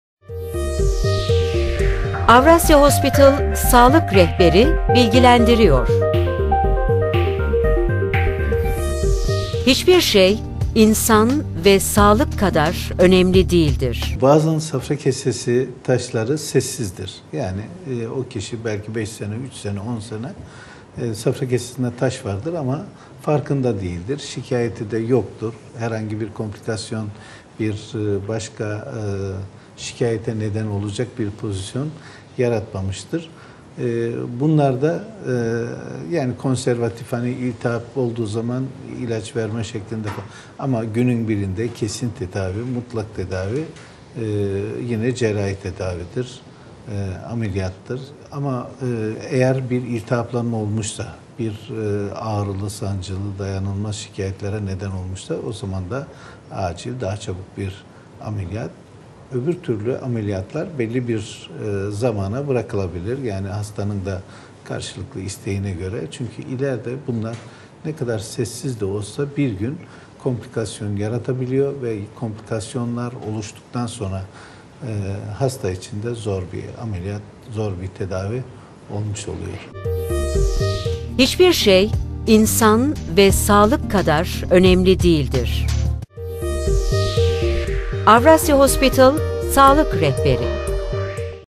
SAFRA KESESİ TEDAVİ YÖNTEMLERİ NELERDİR ? OP.DR. HÜSEYİN URLU ***Avrasya Hospital***
AVRASYA HOSPİTAL GENEL CERRAHİ UZMANI OP.DR. HÜSEYİN URLU
SAFRA KESESİ TAŞI TEDAVİ YÖNTEMLERİ HAKKINDA BİLGİLER VERDİ.